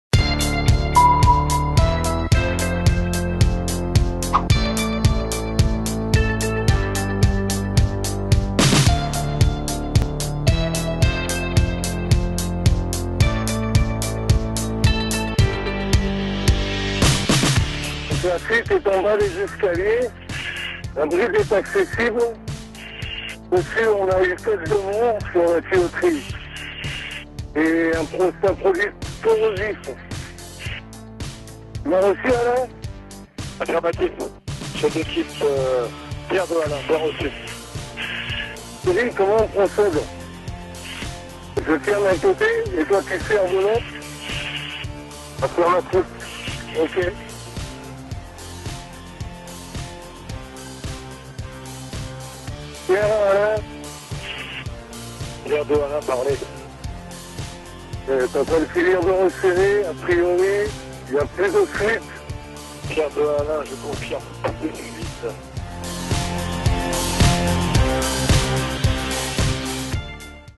APPI-Com sous ARI
Des professionnels dans l’assainissement et maintenance des canalisations en milieu hostile ont utilisé APPI-Com lors de leurs entraînements.

Périphériques audio utilisés :
- Oreillette intra-auriculaire InVisio pour le groupe d'intervention
- Micro casque léger pour le chef d'équipe